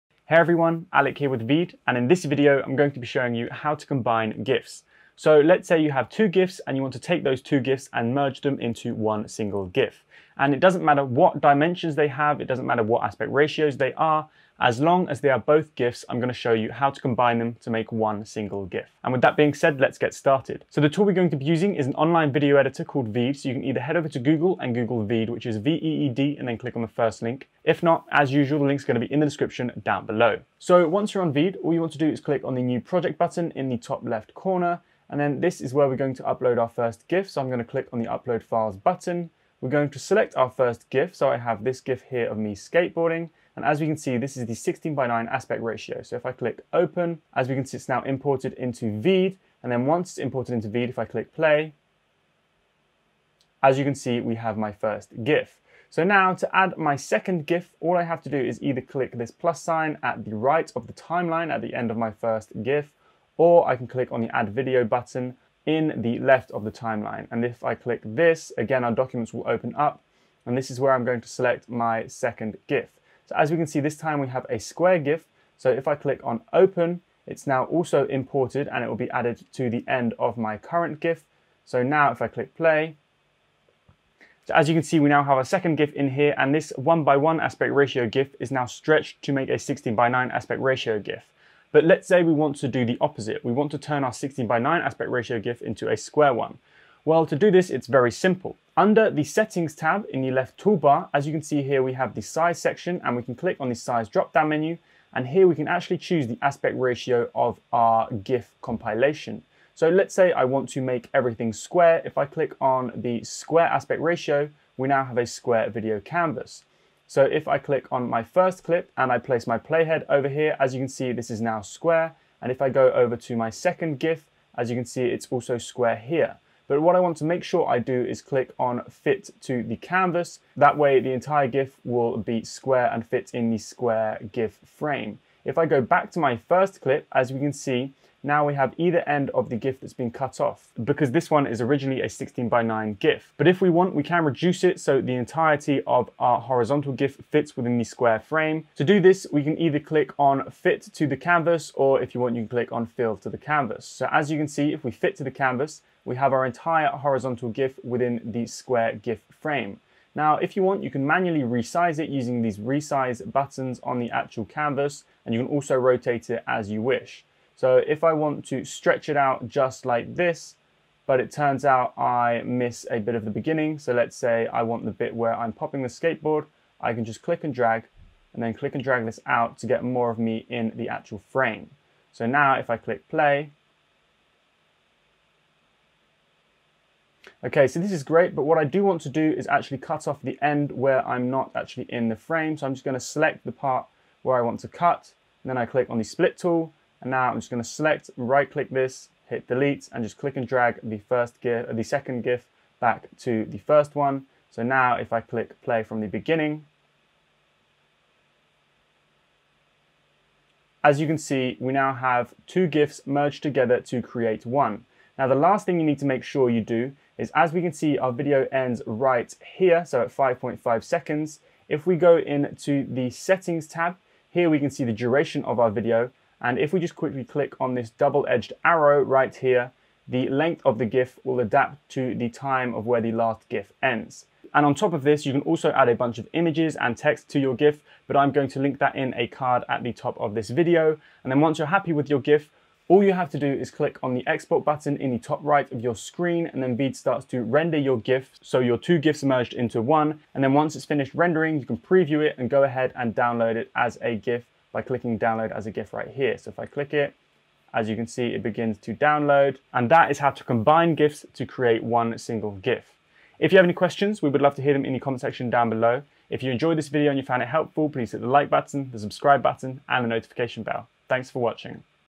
How to Combine GIFS
If you want to combine two or more awesome GIFs into one, watch this full video! In this video, you'll learn how to easily combine GIFs using an online video editor called Veed.

In this video, you'll learn how to combine GIFs online.

If you want to know how to take two GIFs and merge them into one single GIF, no matter what dimensions they have, watch this video!

The first step is to upload your GIF to Veed, an easy-to-use online video editor.

Follow the steps in this video to add your second GIF.

Once your second GIF is added, you can use Veed's video editing tools to change the aspect ratio of your GIF, resize and rotate your GIF, trim your GIFs, and change the duration of your GIF.

Once you finish editing your GIF, export your final GIF and voila!

Timestamps:

00:00 Intro
00:20 Intro to Veed
00:35 Uploading first GIF to Veed
00:56 Uploading second GIF to Veed
01:24 Changing aspect ratio of canvas
02:23 Resizing and rotating GIFs
03:07 Trimming GIF clips
03:35 Changing duration of final GIF
04:01 Exporting final GIF

Good luck!

Alec & VEED.IO